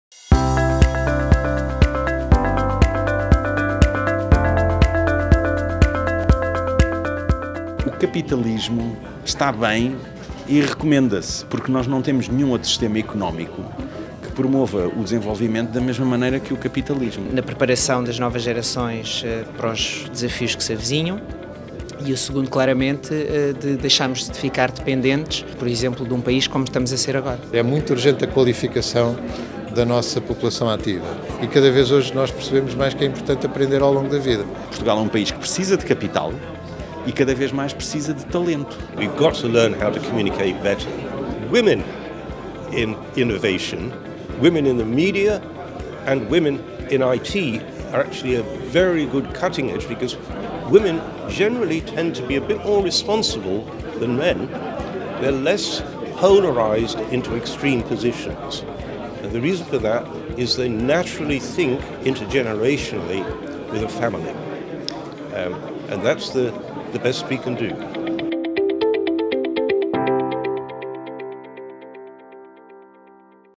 Exclusive Dinner with Sir Paul Collier | O sentido de urgência
Com a presença de Sir Paul Collier em Portugal, a convite da Axians e da APDC, foram apresentadas e debatidas algumas das principais convicções do professor. Num diálogo aberto entre o especialista e Pedro Faustino, Managing Director na Axians Portugal, com cerca de 70 líderes de diversos setores – público, privado e social – foi unânime a urgência de repensar a economia nacional com a sociedade civil.